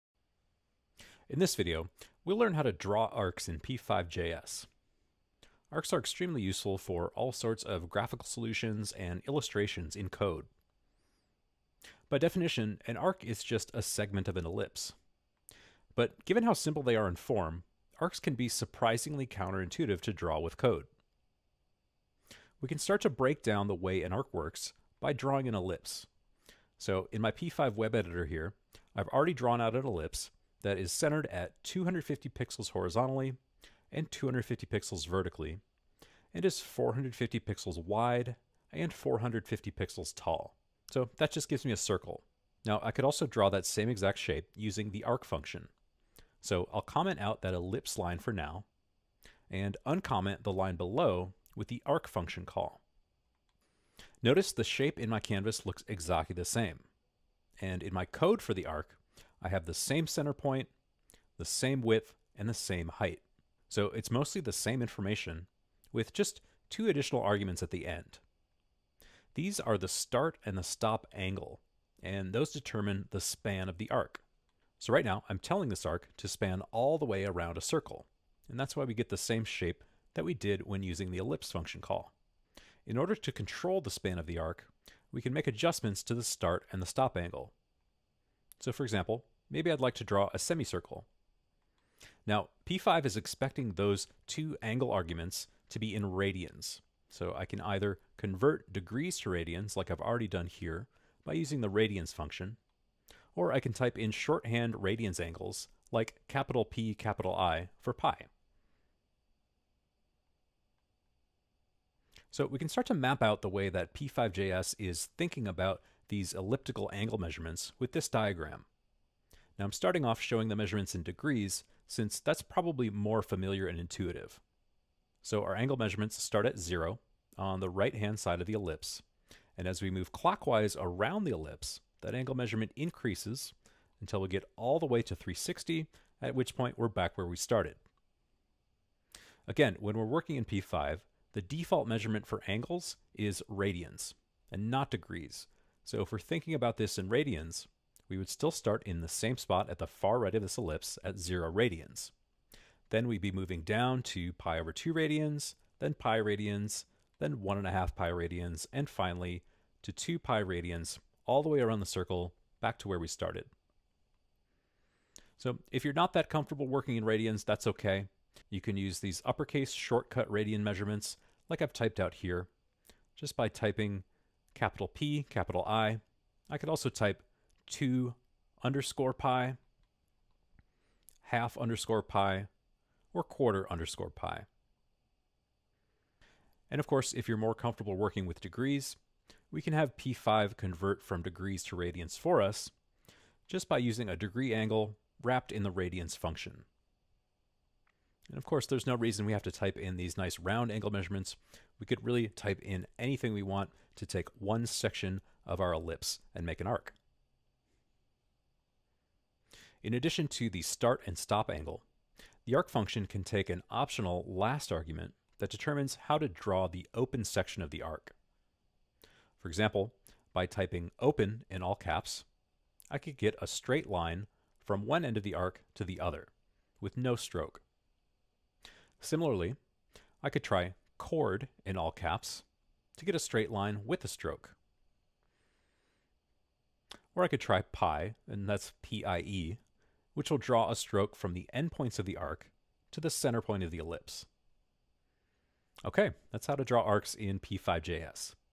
How to Draw Arcs in p5js
Drawing arcs with code in p5js is useful for making all sorts of organic shapes, however using the arc function can be surprisingly counterintuitive. After watching this video, you'll be able to use the arc function to draw an ellipse segment with start and stop angle information in degrees or radians, as well as specify how the open edges of the arc are drawn.